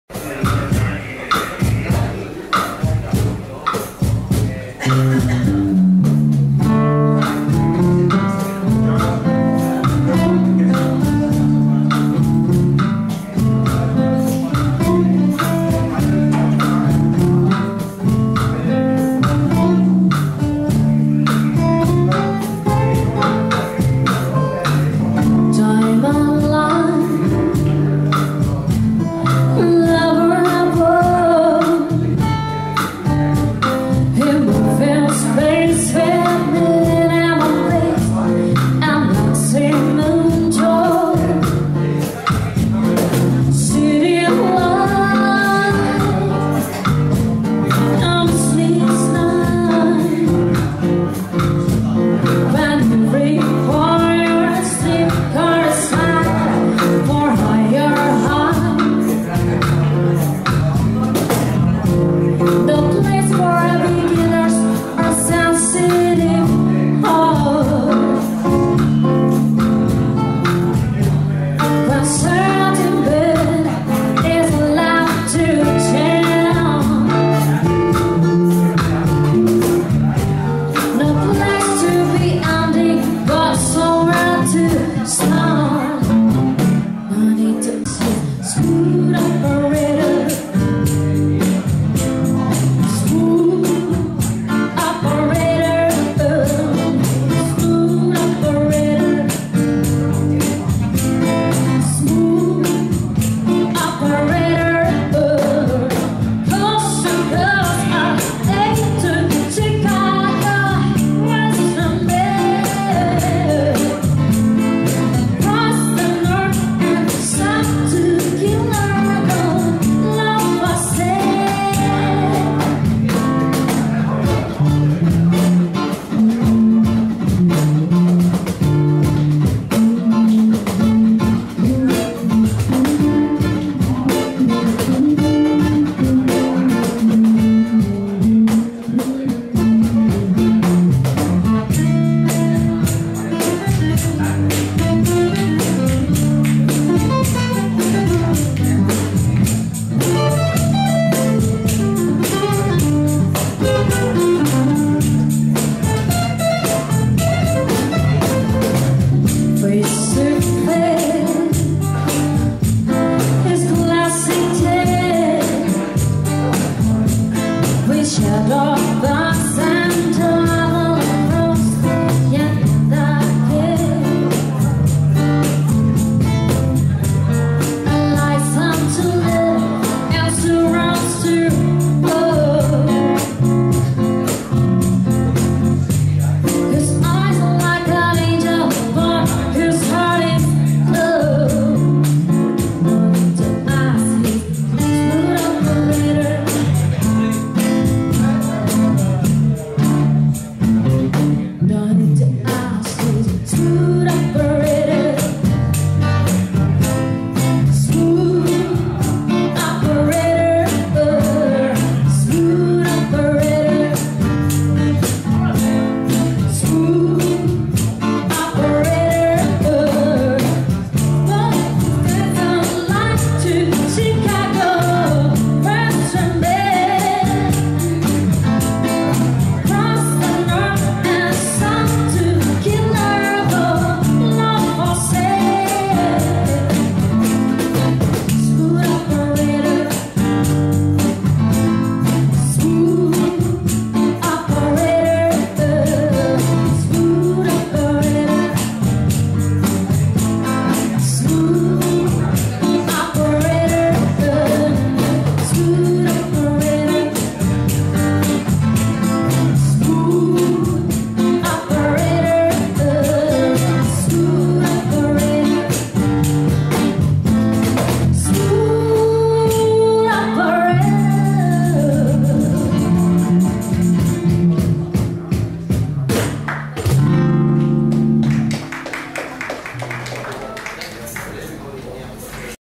band audisi 2# TP Jazz
09 Februari 2014
Band Audisi TP Jazz
at Mirtten Lounge The Papandayan Hotel